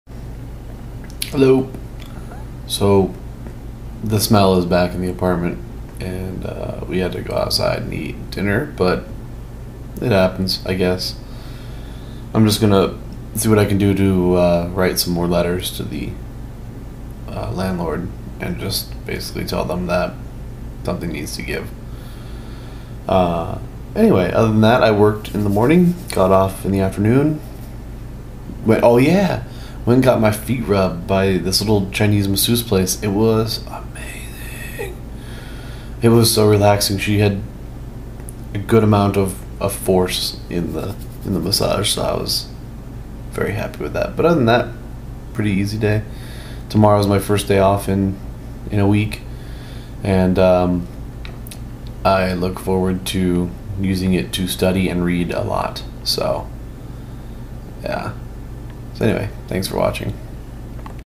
Day 36. Foot Rubbies.
Had my feet rubbed. Nothing special. The smell is back. Whatevs.